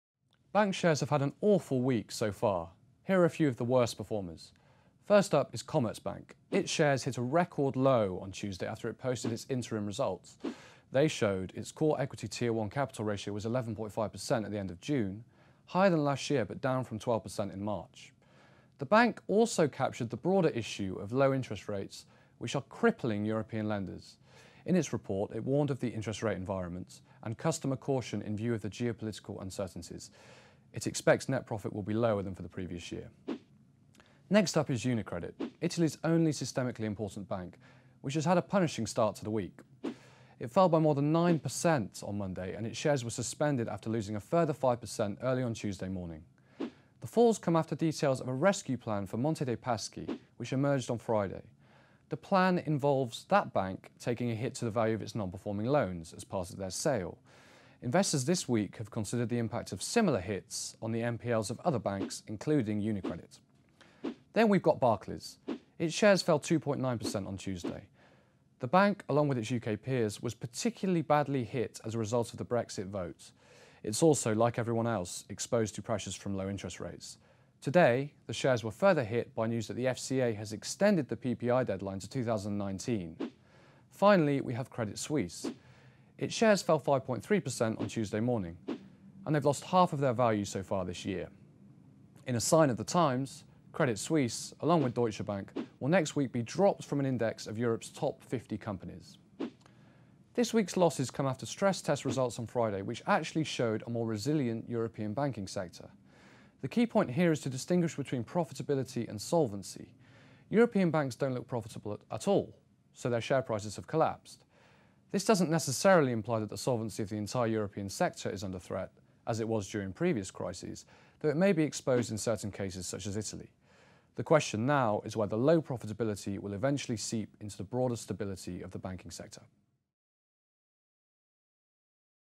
Awful week for European bank shares | FT Markets
Bank shares have had a terrible week, with Commerzbank, UniCredit, Barclays and Credit Suisse among the worst performers. FT capital markets correspondent Thomas Hale looks at whether low profitability will seep into the broader stability of the sector.